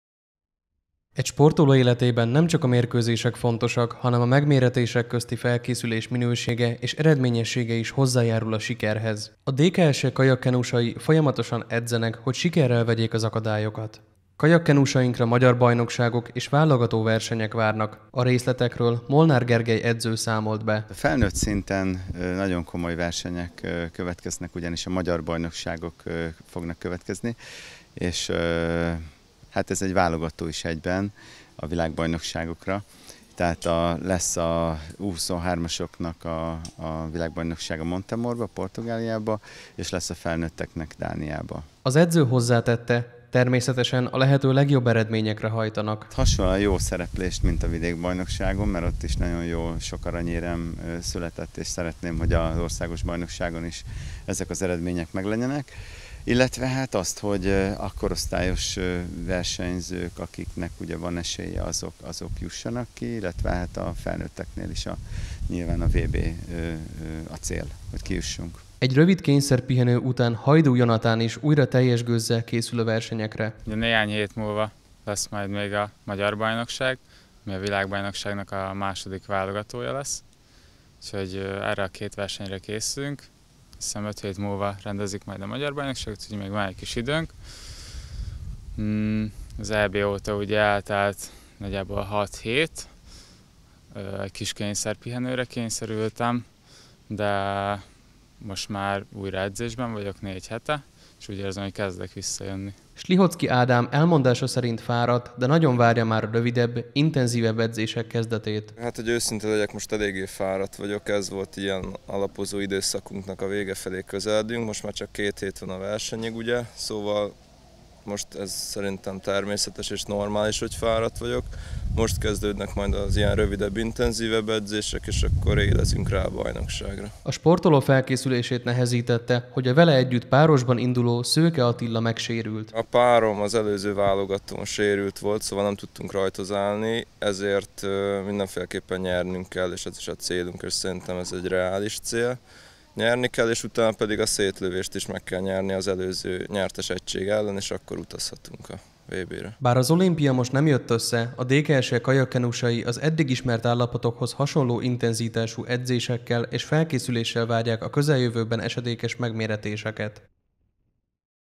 DSTV | KÉSZÜLNEK A KAJAK KENUSOK
Folytatják a felkészülést a sportolóink. A magyar bajnokságokra, válogató versenyekre hangolódnak a DKSE kajak-kenusainak utánpótlás és felnőtt versenyzői.